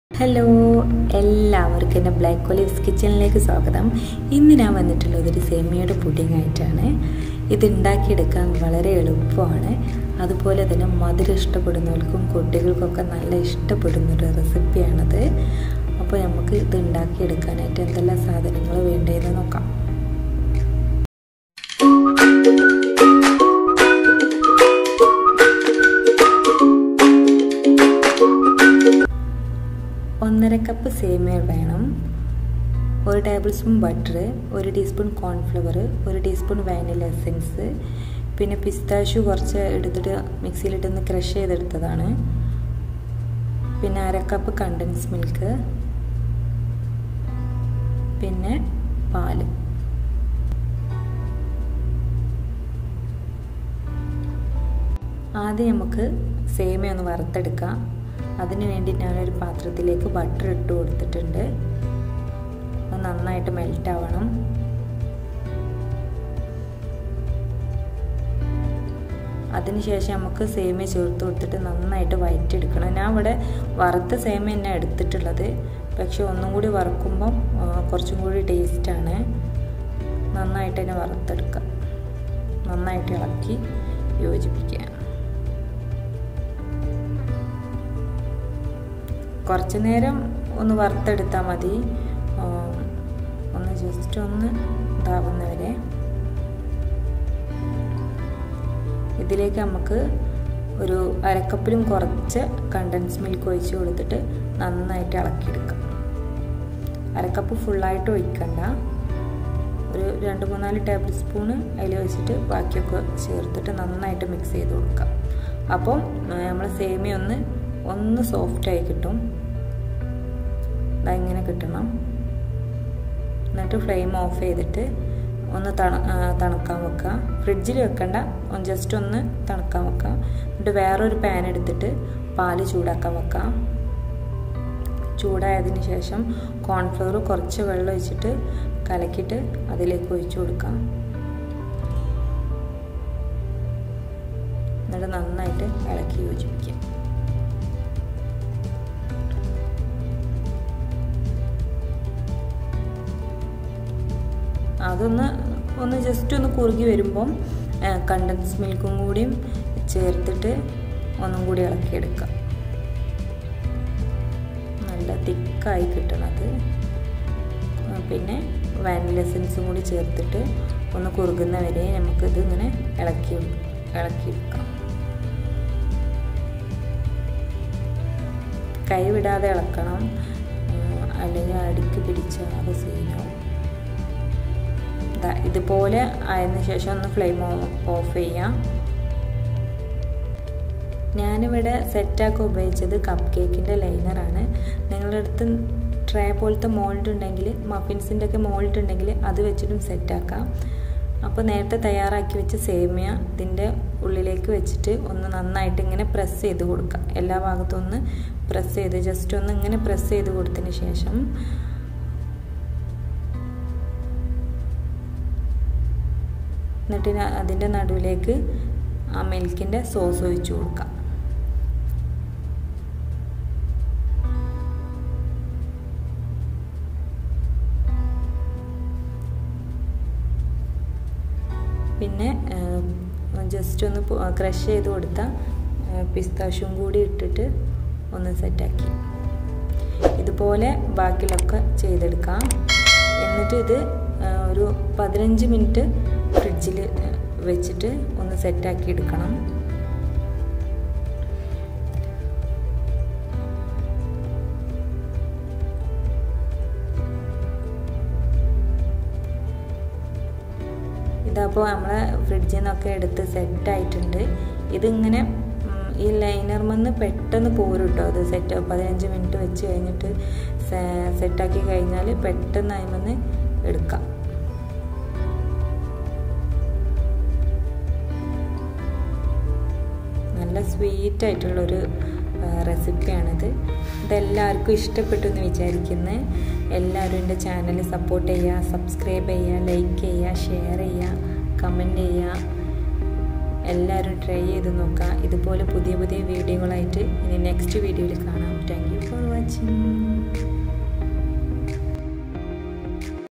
Simple semiya pudding |എളുപ്പത്തിൽ എങ്ങനെ സേമിയ പുഡിങ് ഉണ്ടാക്കാം
Ingredients :-
Semiya  1 1/2 cup
Condensed milk 1/2 cup
Cornflour 1tsp
Vanila extract 1tsp
Milk 1/2
Butter 1tsp
Crushed pistachio

Base Preparation:-

Heat the bowl, Add butter then add vermicelli then sort  it well. Add condensed milk cook with low flame,  then keep it aside.

Sauce Preparation :-

Heat the Pan add milk, upon boiling the milk add corn flour the Add condensed milk ,add vanilla essence until the batter become creamy consistency. And freeze the pudding for 15 minutes..

Semiya Milk Pudding is ready... Enjoy...😋